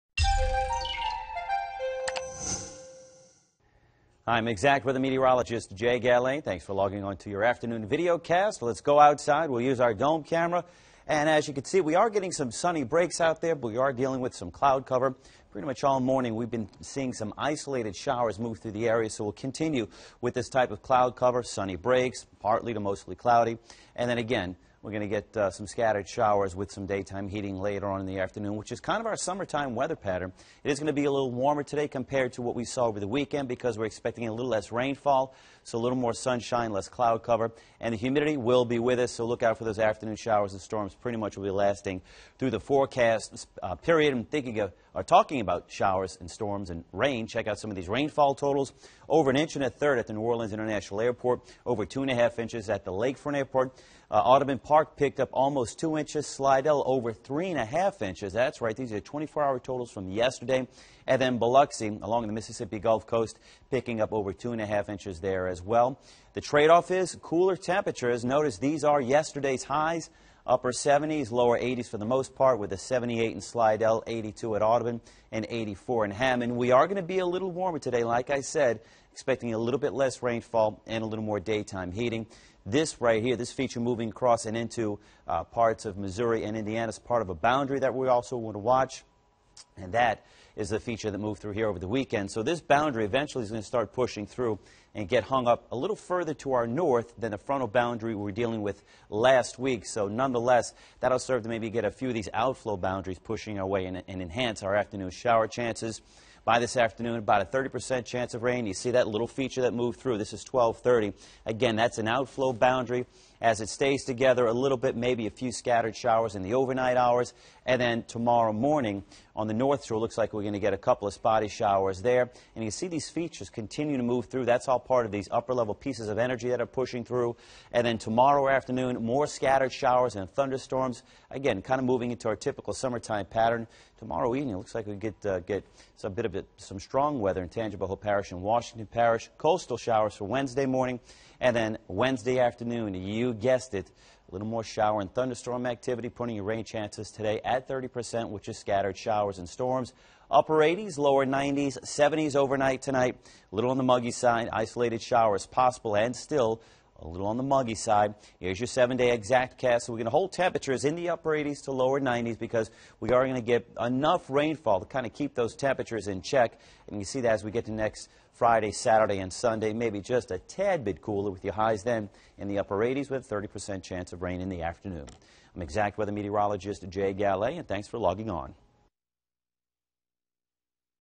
More afternoon rain with slightly warmer temperatures
It'll be another day of scattered showers and storms as afternoon temperatures get a little warmer.